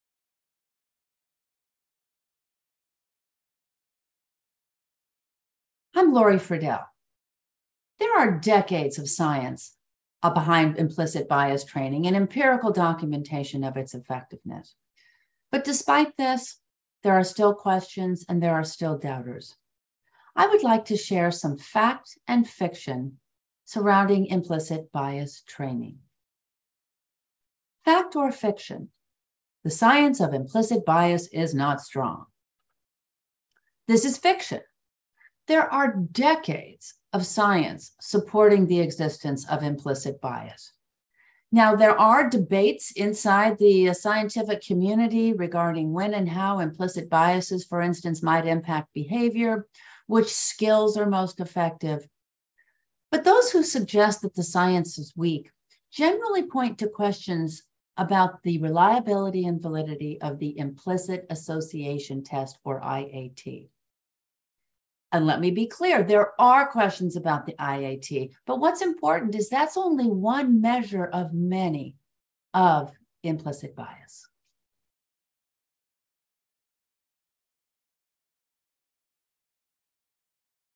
Fact or Fiction? A Series on Implicit Bias - Part 1
In this six-part series, Dr. Lorie Fridell, CEO of Fair & Impartial Policing, tackles some of the common misperceptions about implicit biases, translating some of the complex science into plain language. She identifies the facts and fictions surrounding implicit biases and the importance of implicit-bias-awareness training.